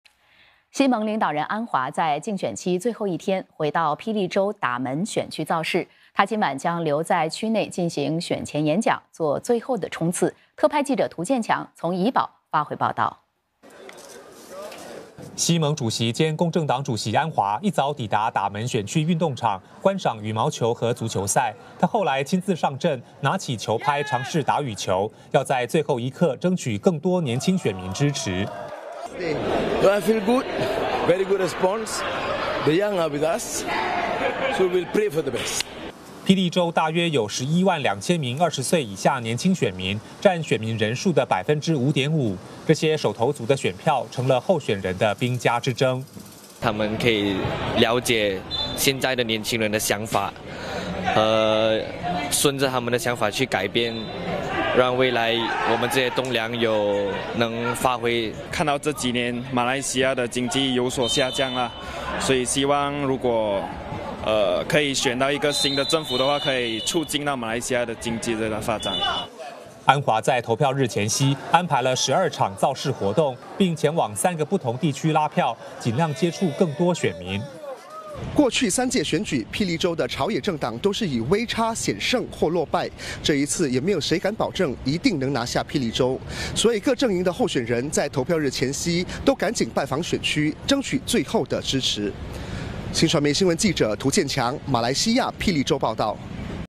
安华竞选期最后一天回到霹雳州打扪选区造势
希盟领导人安华在竞选期最后一天回到霹雳州打扪选区造势。他今晚将留在区内进行选前演讲，做最后冲刺。